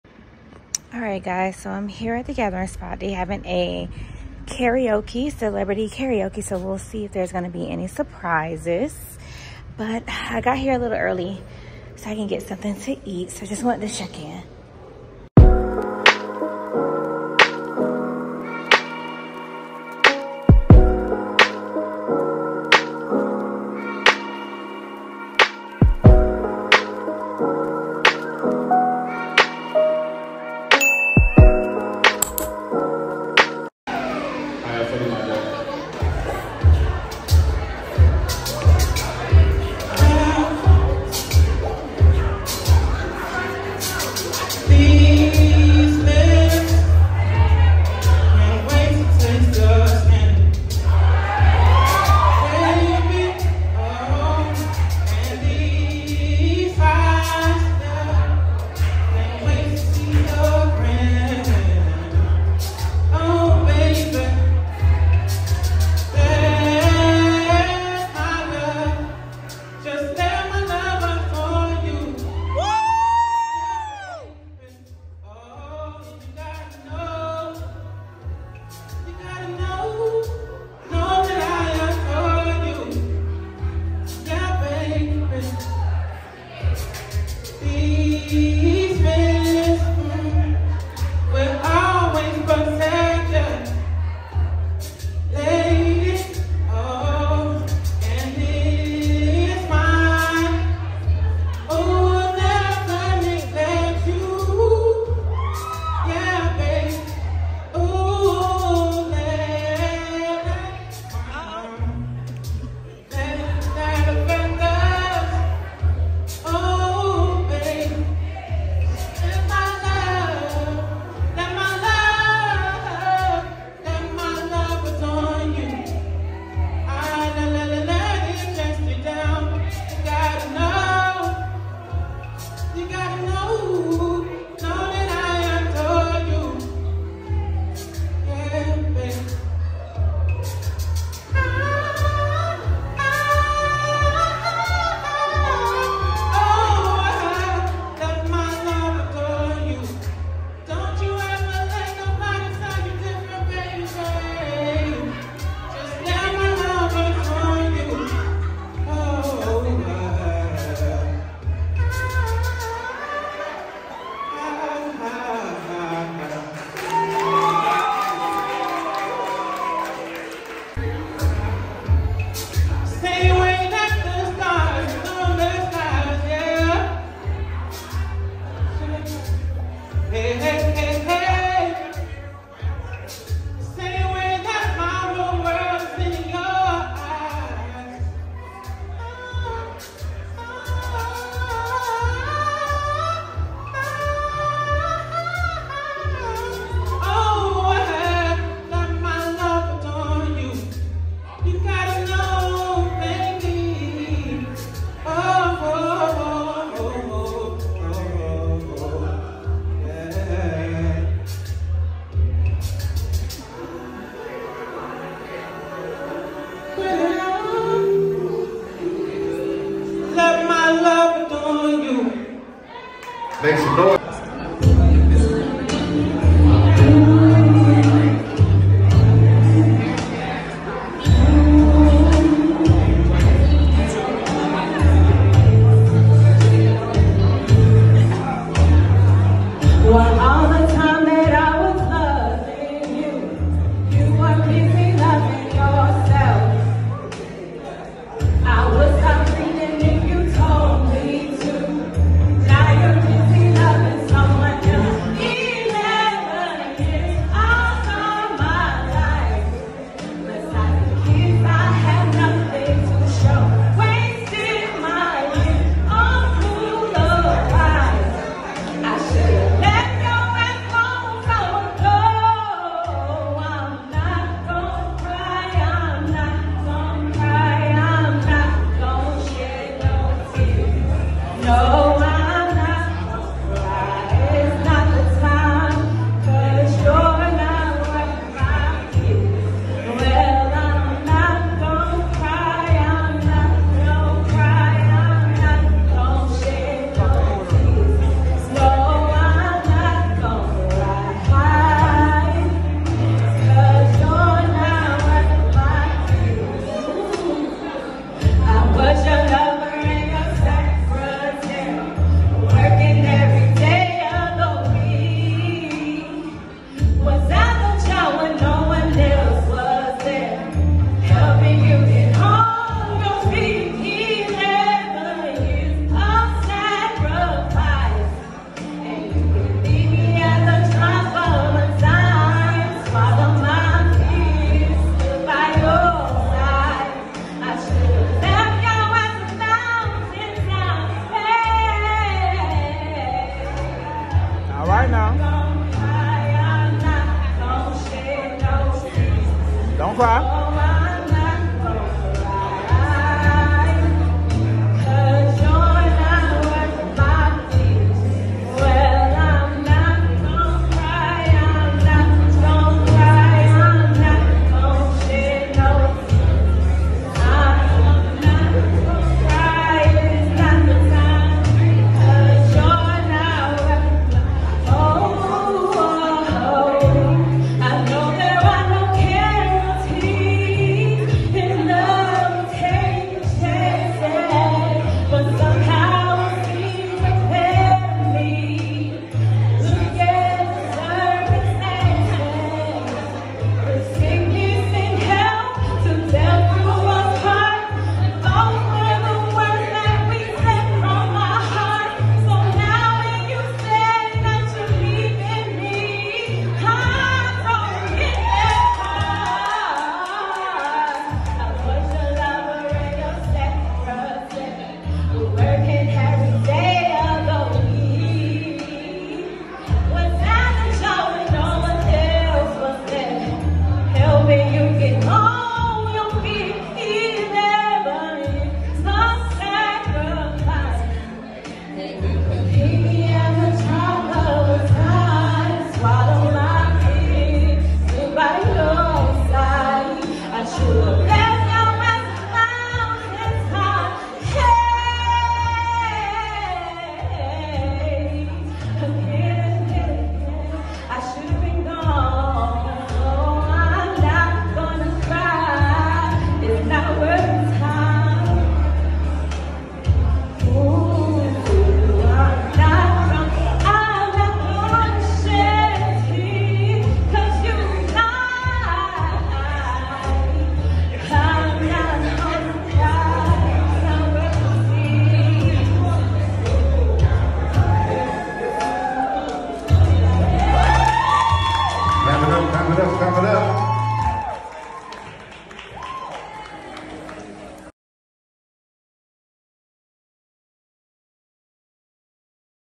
Fun Night Out in Atlanta! See What Happened at The Gathering Spot's Karaoke Night! | Miguel | Mary
Weekend Vlog. Join us for karaoke night!
00:00 Intro
00:34 Devo Karaoke set
03:44 Meka Karaoke set

♡ FAQs ♡
Age: 35 & 30
Location: Atlanta
Sign: Scorpio & Cancer

*Some links maybe affliate links. Meaning we may receive payment if you purchase any products.